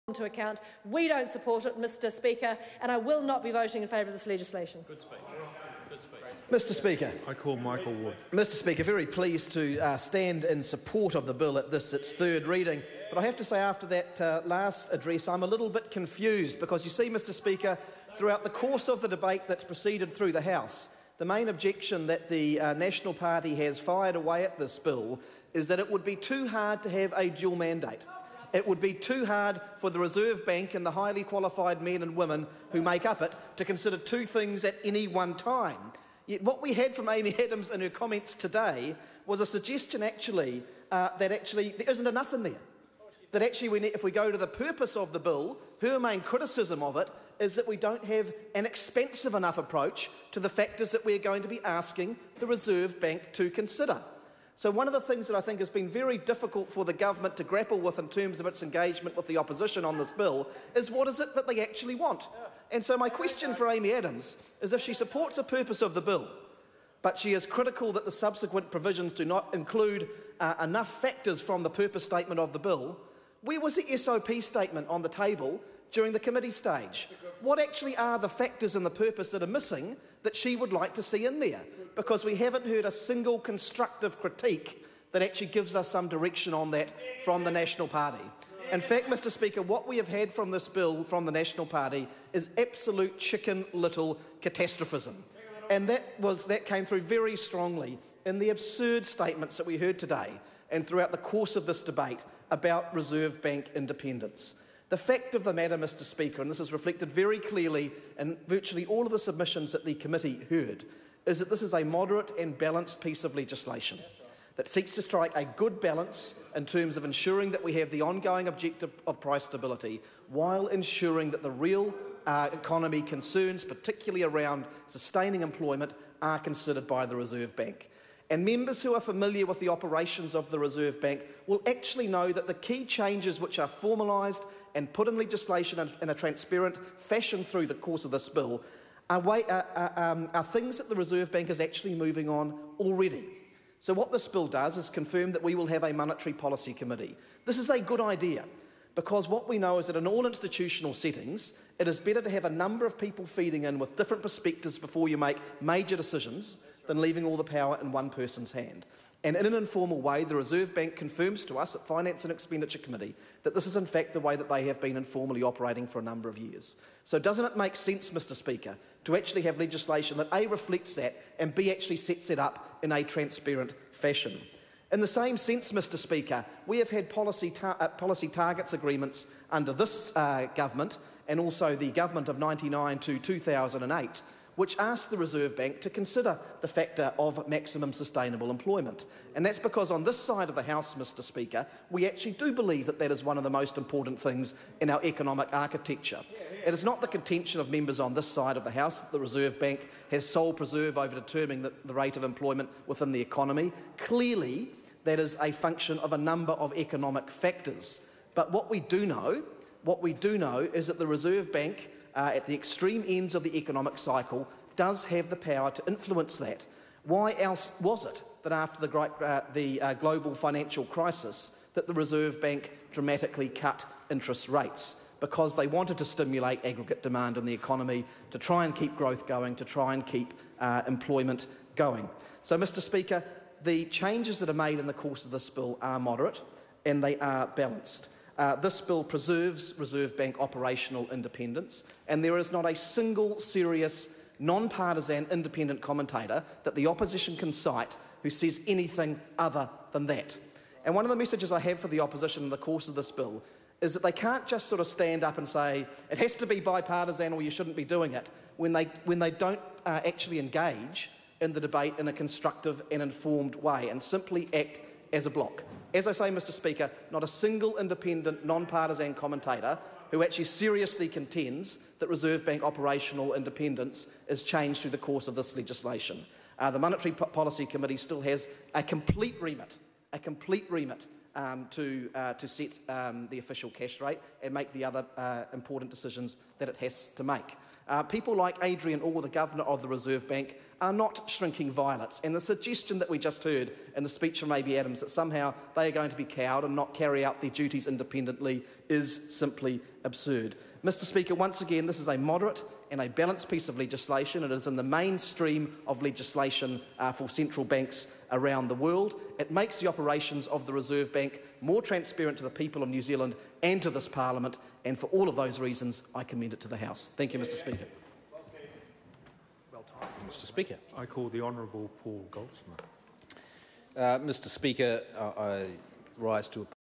Reserve Bank of New Zealand (Monetary Policy) Amendment Bill - Third Reading - Video 3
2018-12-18 - Reserve Bank of New Zealand (Monetary Policy) Amendment Bill - Third Reading - Video 3

Michael Wood